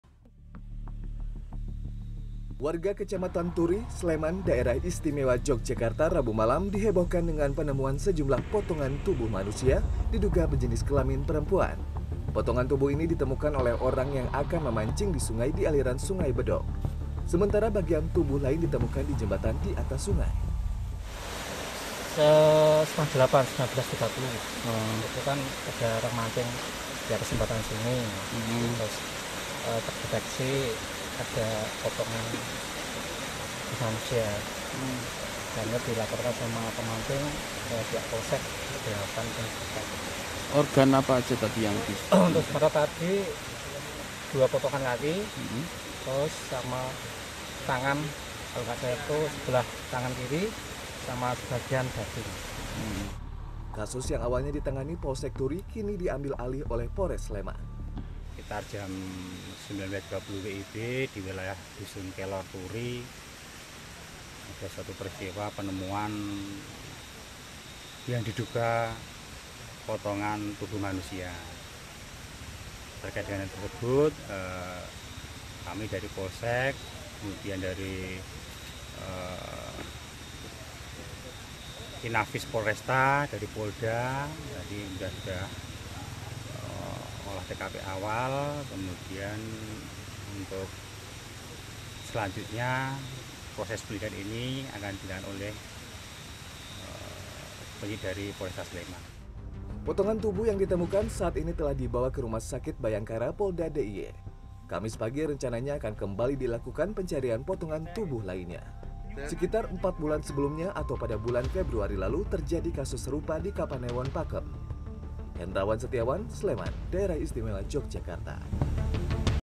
Penemuan Potongan Tubuh Manusia Diduga Korban Mutilasi
Dugaan kasus mutilasi terjadi di Kapanewon Turi, Kabupaten Sleman, Daerah Istimewa Yogyakarta.
Sekitar empat bulan sebelumnya, terjadi kasus serupa di kecamatan lain.